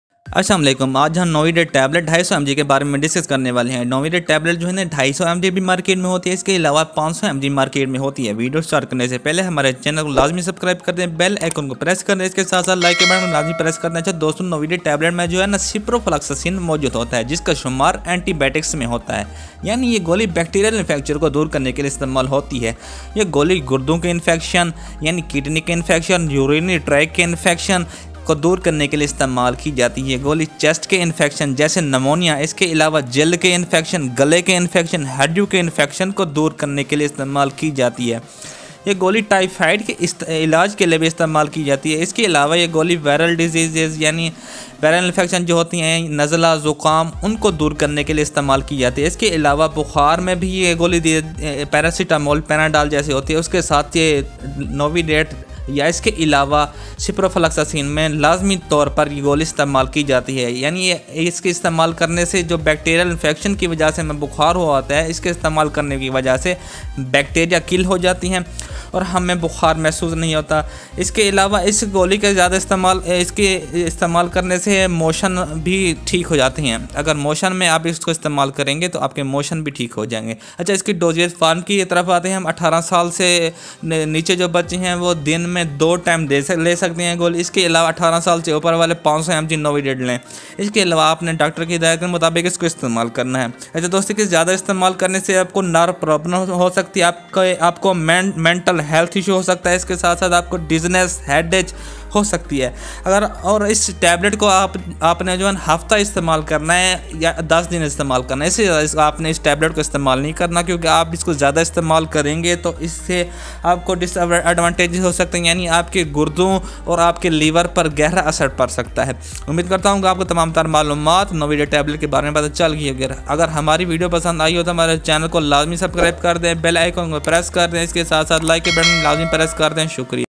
Tablet Novidate 250mg,500mg uses,dose and disadvantages in urdu/hindi||ciprofloxacine||Antibiotic
Description
1.tablet used for bacterial imfection
2.used for kidney,throat and viral infection
3.Medic official